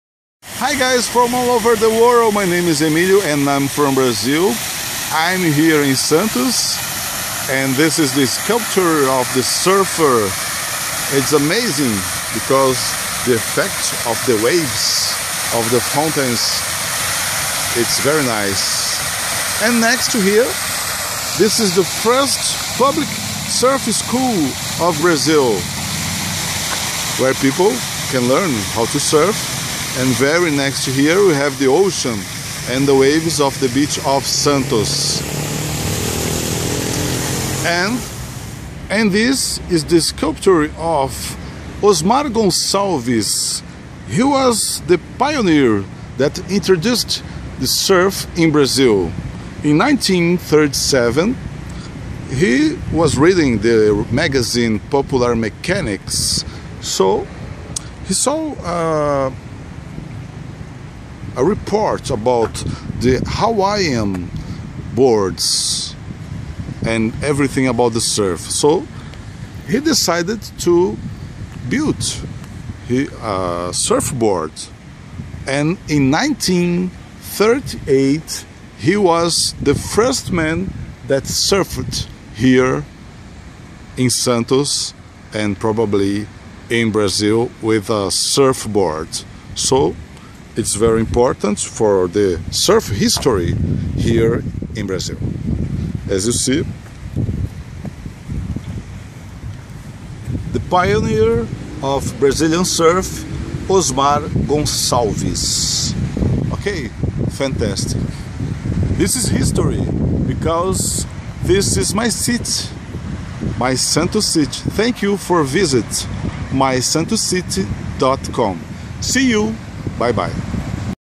Talking about the first surfer of Brazil
Osmar Gonçalves was the first surfer of Brazil